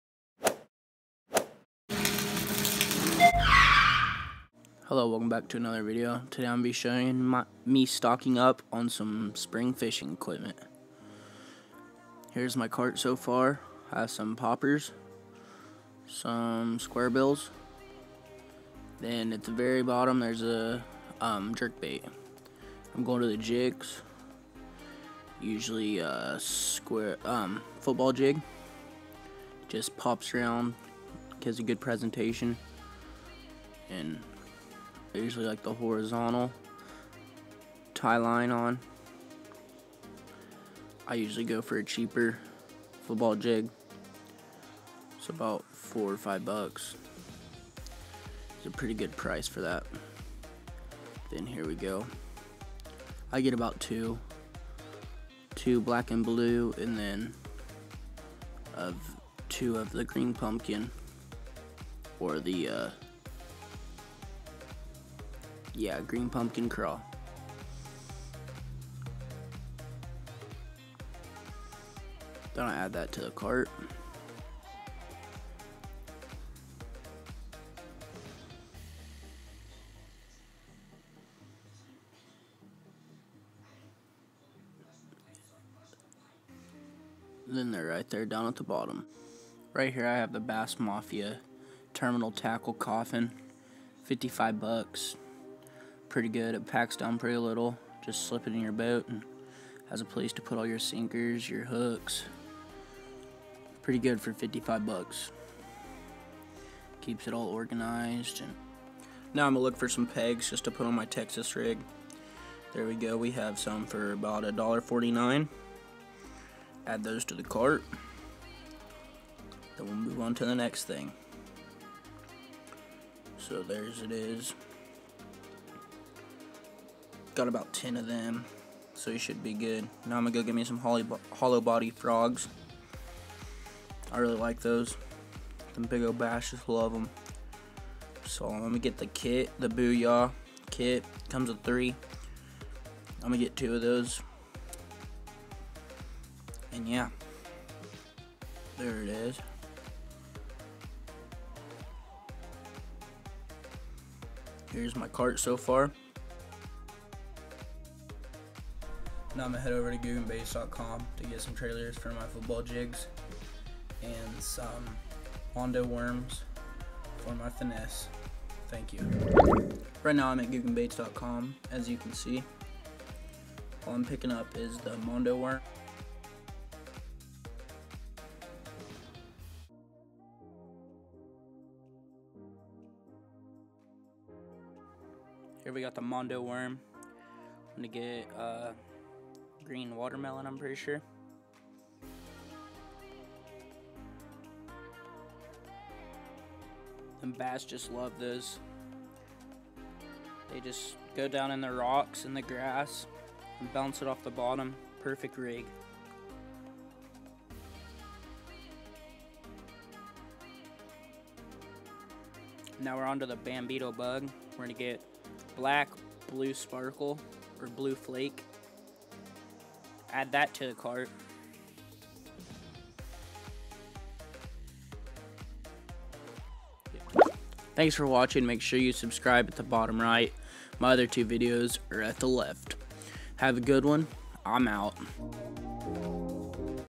new spring tackle from tackle warehouse and googan baits. :)
I DECIDED TO ADD SOME TACKLE TO THE COLLECTION.